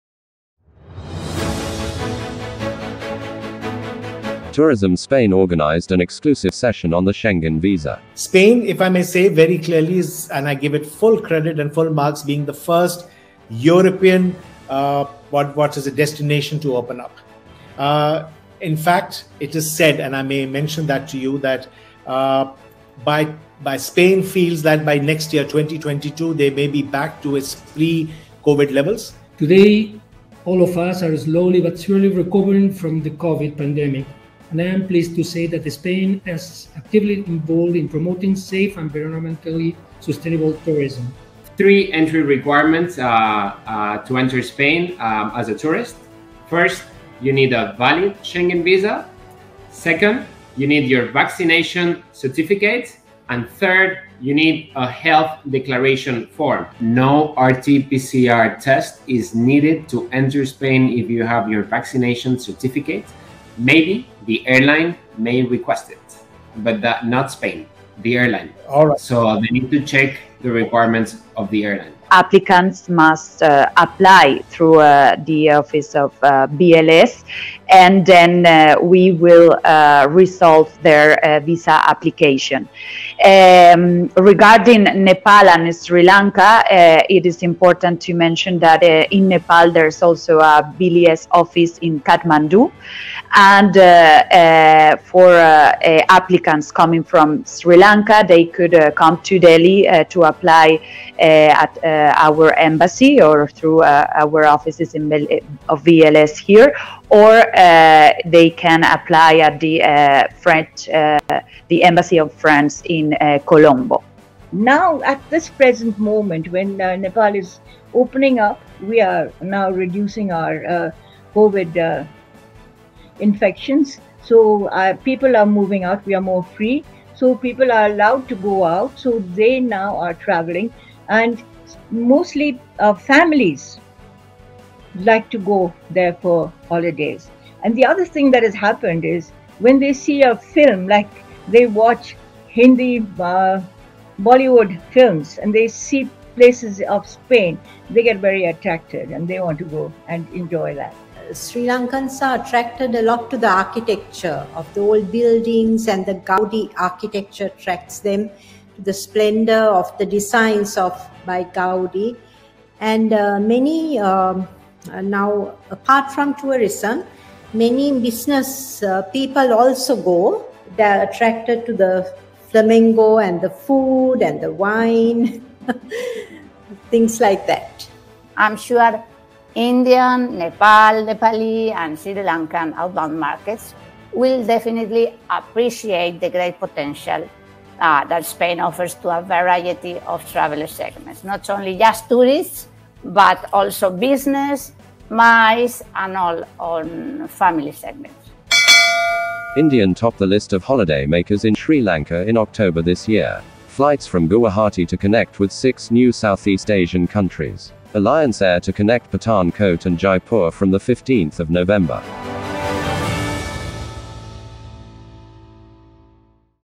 Episode 1482TravelTV.News - Episode 1482 (Nov 11, 2021)
- In this episode TravelTV.News focuses on:

- Tourism Spain organized an exclusive session on the Schengen Visa
- Indian topped the list of holiday makers in Sri Lanka in October this year
- Flights from Guwahati to connect with six new Southeast Asian countries
- Alliance air to connect Pathankot and Jaipur from 15th November

- We Interviewed:

- SanJeet, Founder and Managing Director, DDP Group
- JOSE MARIA RIDAO, Ambassador, Embassy of Spain in India
- FERNANDO HEREDIA NOGUER, Consul General, Embassy of Spain in Mumbai
- HELENA ESCORIAL LOPEZ, Head-Consular Section , Embassy of Spain in India
- AMBICA SHRESTHA, Honorary Consul, Consulate of Spain Kathmandu, Nepal
- PRIYA JAYAWARDENA, Honorary Consul, Consulate of Spain in Colombo, Sri Lanka
- ELISA ROBLES-FRAGA, Director, Tourism Office of Spain in India